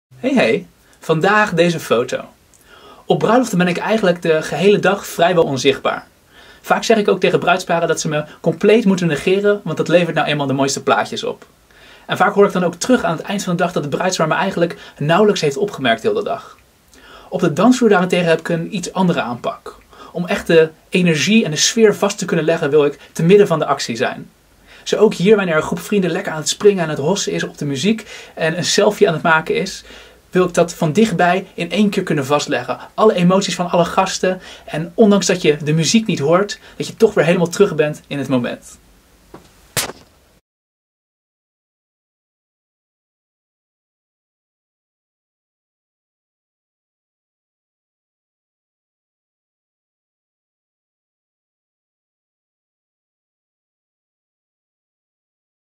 Trouwfotograaf Rotterdam Het Verhaal Achter ep14
Marnix de Stigter Bruidsfotograaf. Deel 14 van "Het Verhaal Achter"
Korte vlogs over foto's die ik bij afgelopen bruiloften heb geschoten waarin ik vertel over hoe we ze gemaakt hebben.

Trouwfotografie Rotterdam Zuid Holland